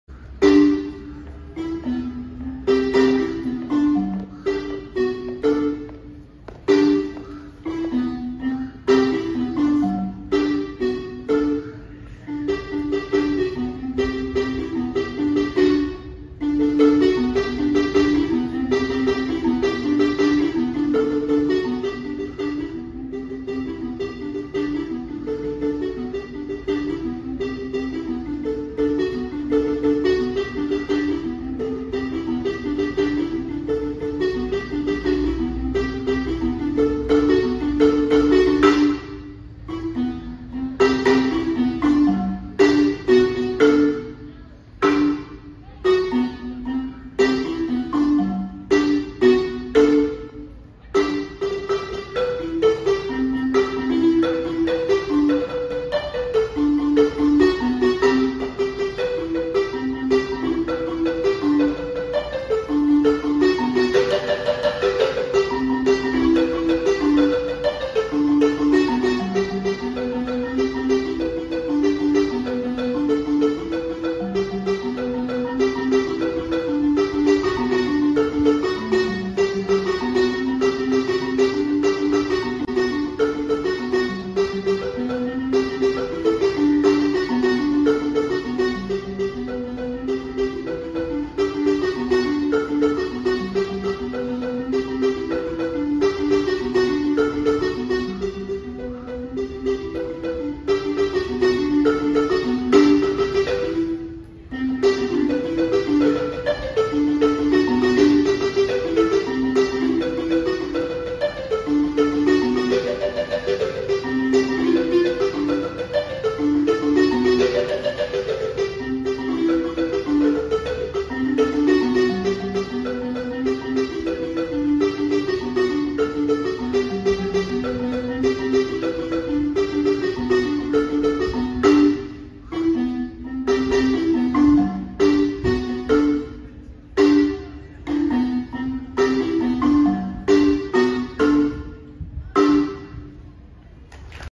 #365 days of marimba day 82 - Solo by Kuda Mudehwe
Welcome to my quest of giving you 365days of pure Zimbabwean Marimba! ENJOY